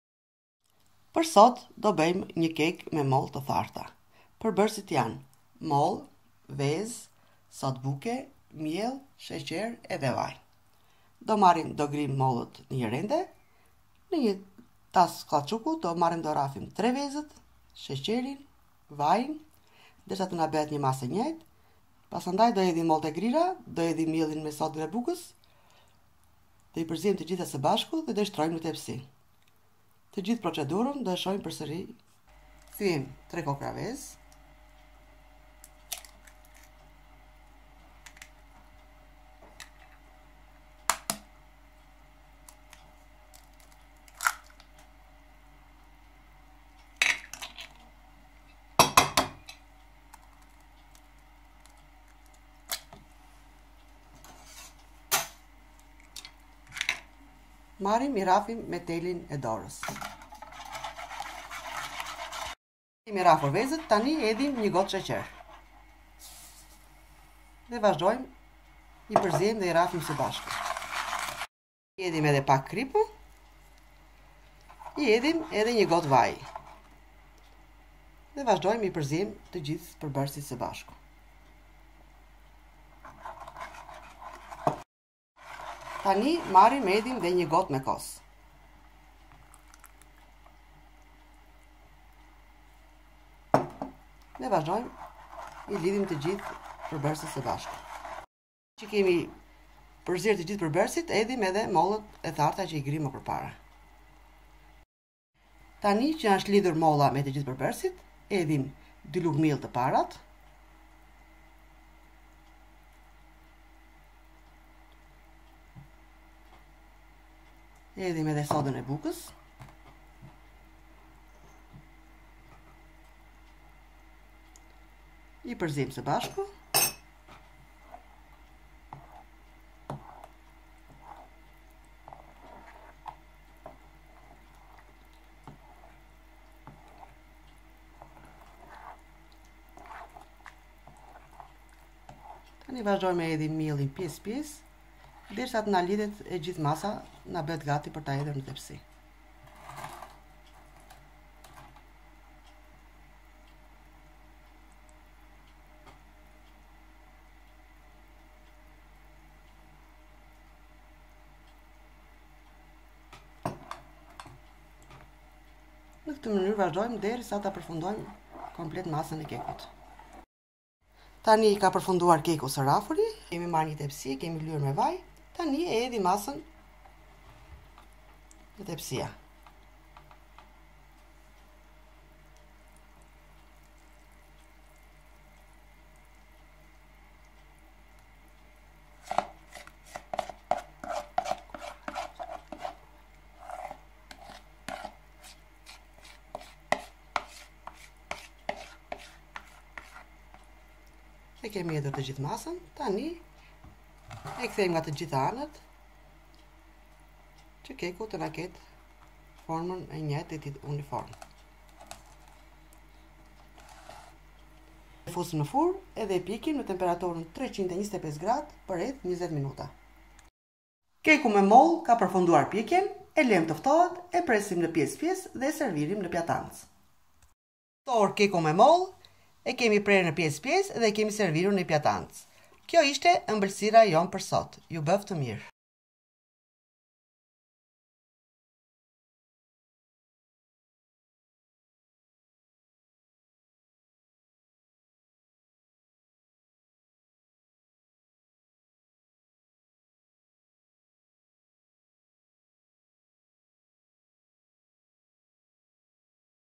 Kek me molle
Receta
Molle te tharta
SHeqer
Vaj gjelle
Miell
Kos
Sode buke
Veze